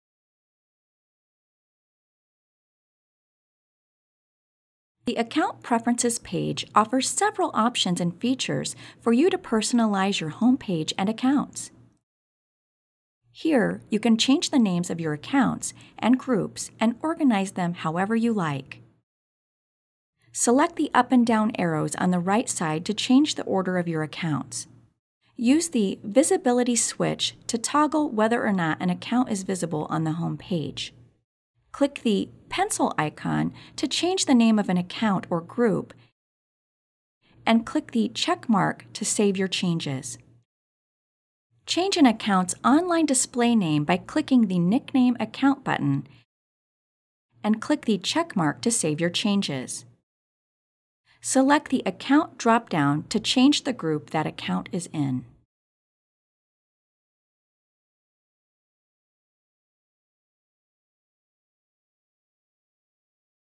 OlyFed Digital Banking // Account Preferences
The Account Preferences page offers several options and features for you to personalize your home page and accounts.